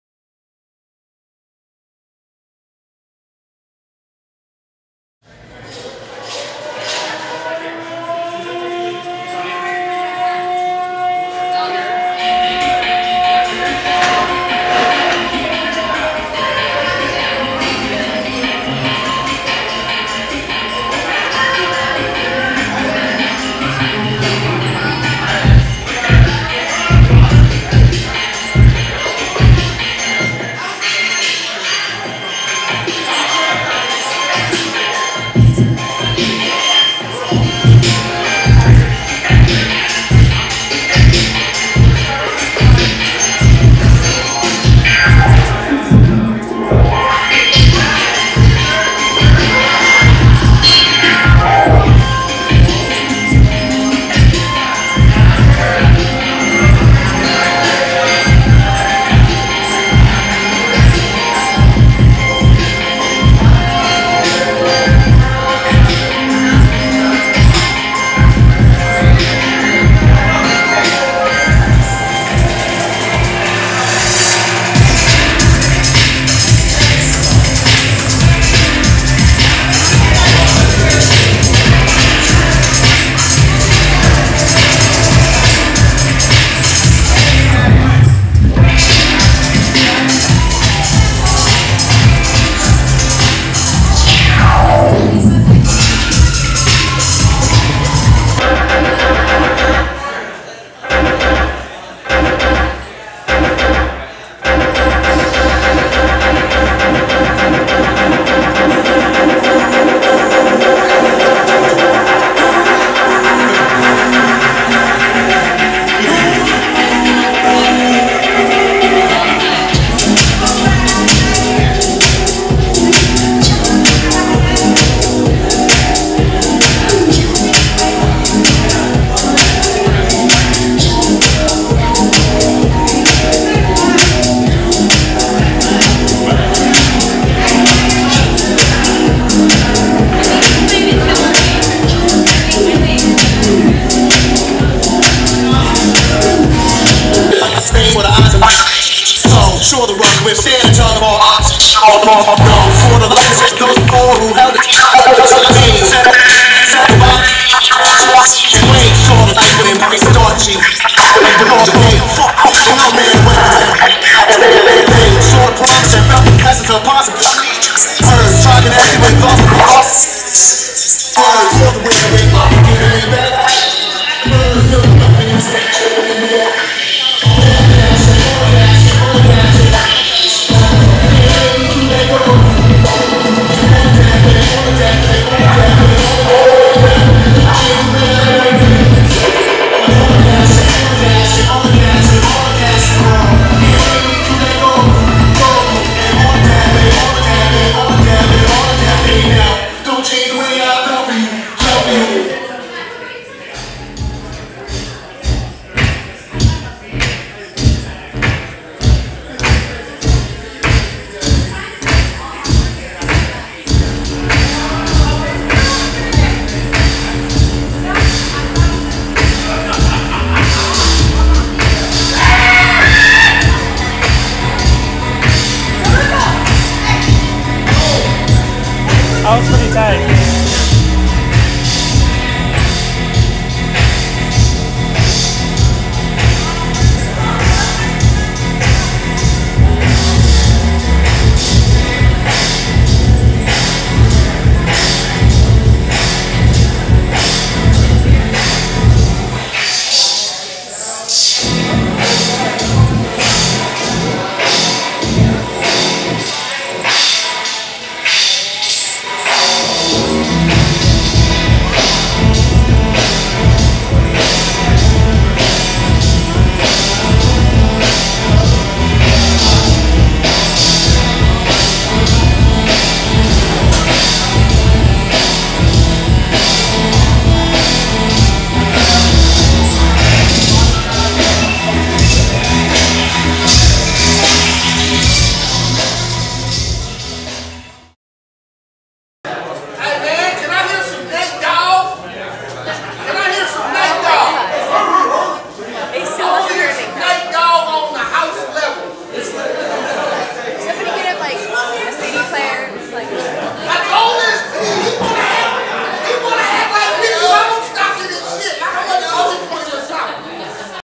Jason Perez - Atropos Films - Hang the DJ - Live at O'Sullivan's 3/31/11
A compilation of Jason Perez's show, playing at O'sullivan's as part of the hang the DJ crew.
Filmed by Atropos Films.